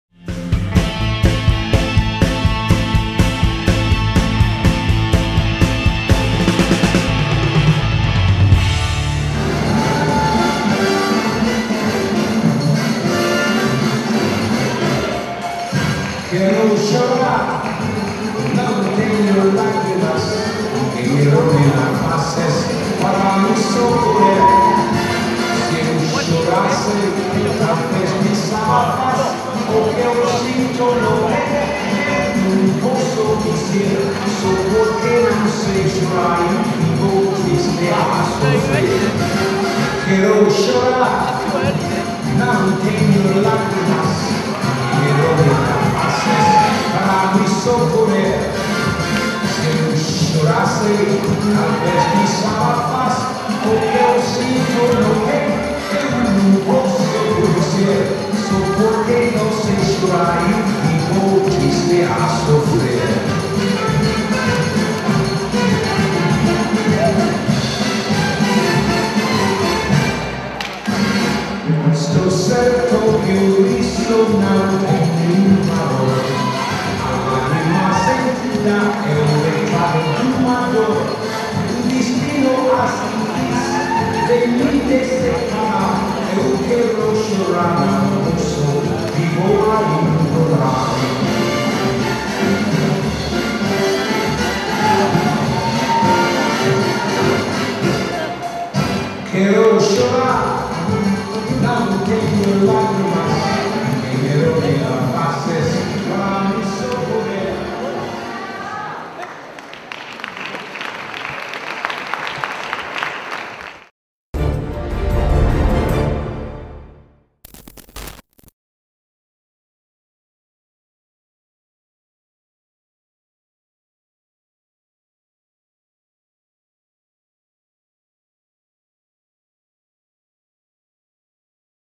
Mikadze - Novikova, RUS | 2014 GS LAT Stuttgart R2 S | DanceSport Total
Nikolay Mikadze - Yana Novikova, RUS, dance the second round Samba of the 2014 WDSF GrandSlam Latin in Stuttgart on August 14.